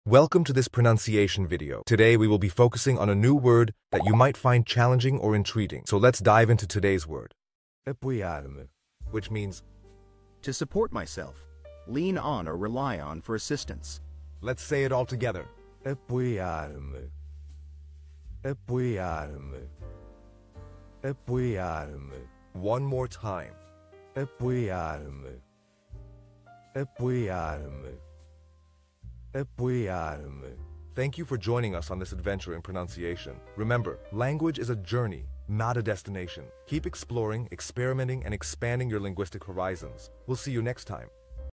How to pronounce Apoiar-me
Master the Pronunciation of 'Apoiar-me Which Means support me' - which means : To support myself;Lean on or rely on for assistance. 📝 with @PortuguesePronunciation 🎓🗣️ - [Your Guide to Portuguese]

Learn to pronounce 'Apoiar-me' perfectly in Portuguese with our easy and engaging tutorial by @PortuguesePronunciation! 📖📽️ In this comprehensive video, we provide you with native speaker examples 🗣️👂 and phonetic breakdowns 🎵 to ensure you get the pronunciation just right.

How to say Support me in Portuguese